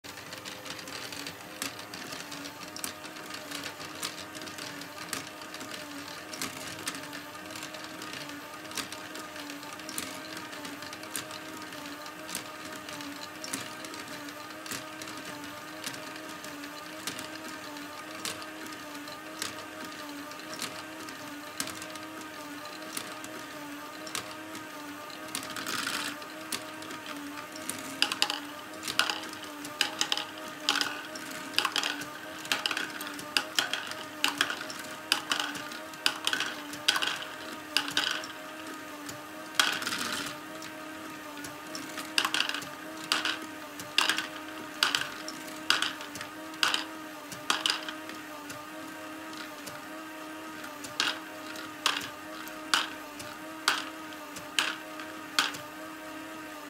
Lego GBC Shared Power modules
Three modules (Ball Pump, Batch 13, Pinwheel) by Sam Friesen running using shared power. Means only one motor is used. The motor sits in the middle module.
This is the best combination when using these 3 modules.